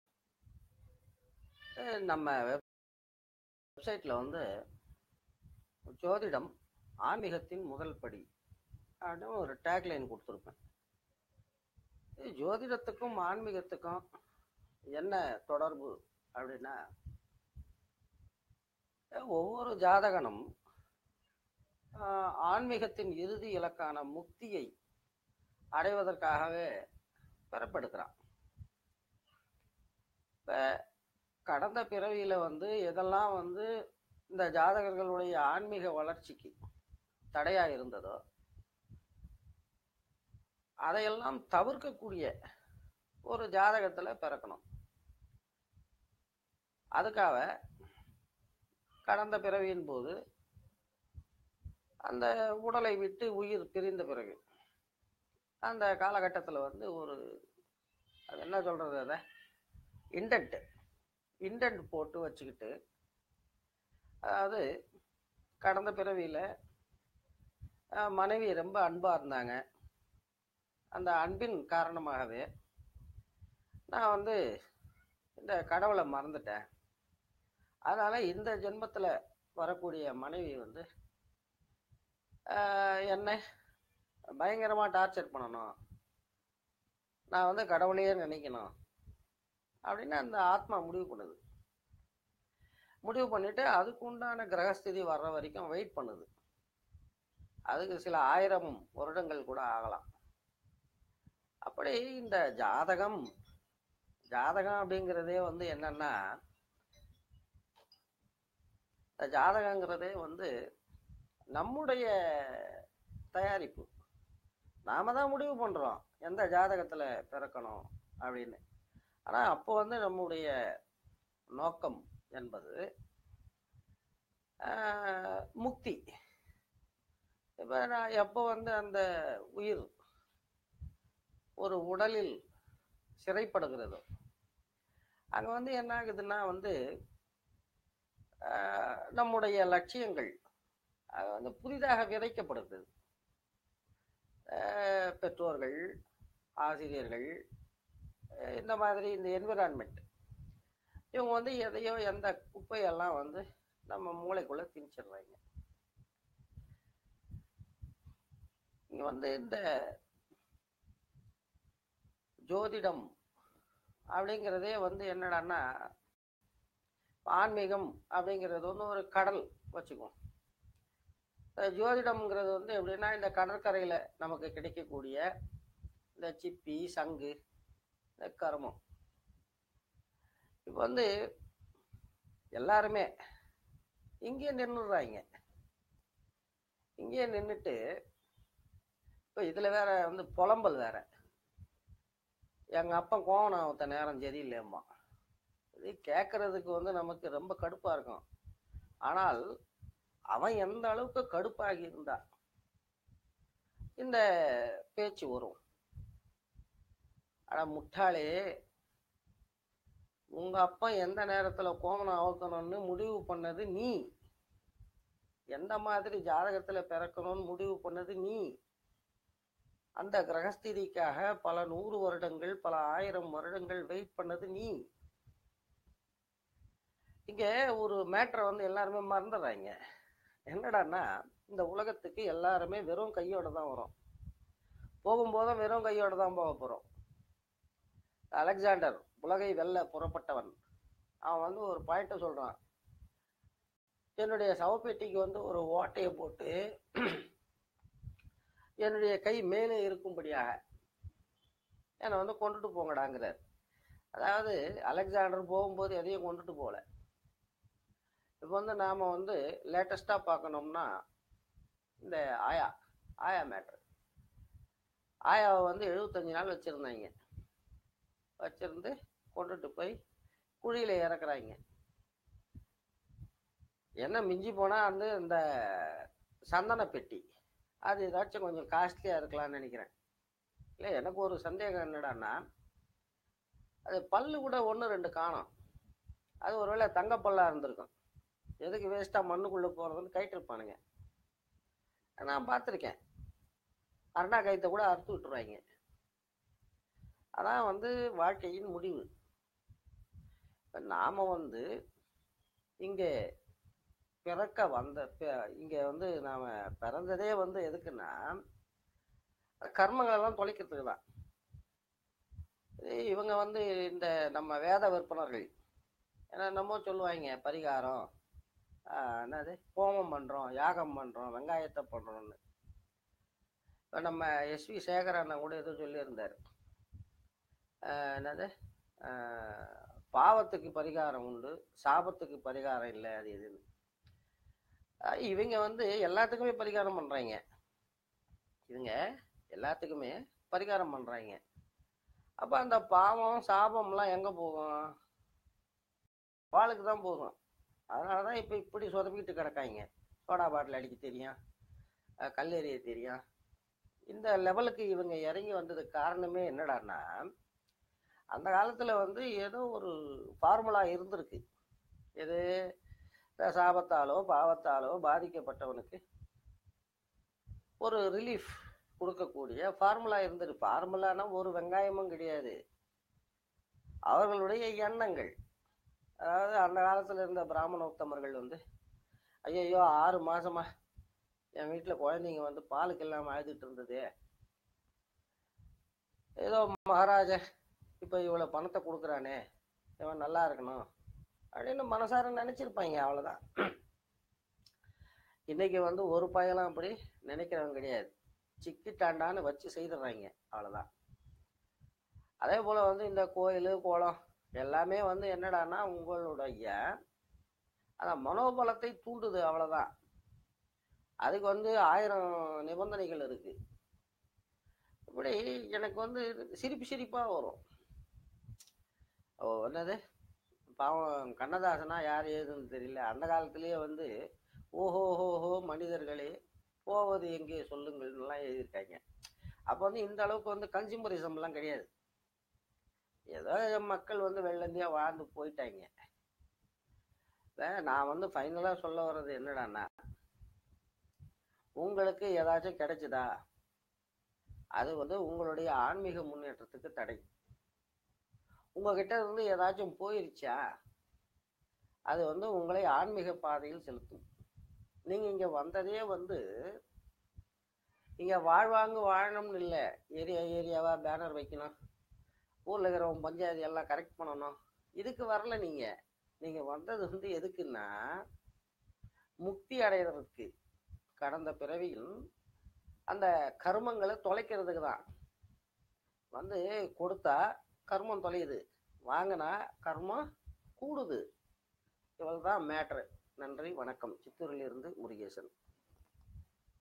உங்க ஜாதகத்தை முடிவு பண்ணதே நீங்க தான் பாஸ் !
உங்கள் ஆசை என்பது உங்களை பொருத்தவரை நியாயமானதா கூட இருகலாம்.
ஆனால் உங்க ஜாதகத்துல உள்ள கிரகஸ்திதிக்கு அது பேராசையா
இருக்கலாம்.
பேராசை பெரு நஷ்டம்.
உங்க ஜாதகம் உங்க ப்ரப்போசல் தான். ப்ரப்போஸ் பண்ணும் போது ஆன்ம ரூபமா இருந்திங்க.
உங்க நோக்கம் முக்தி.
இப்ப உடல் என்ற சிறையில் இருக்கிங்க. உங்க நோக்கம் நோக்கம் புக்தி ( சர்வைவல்)
இதுதான் எல்லா அதிருப்திகளுக்கு மூலம். ரோஜாவாய் பிறக்க வரம் வாங்கி விட்டு முட்களுக்காக முனகினால் ??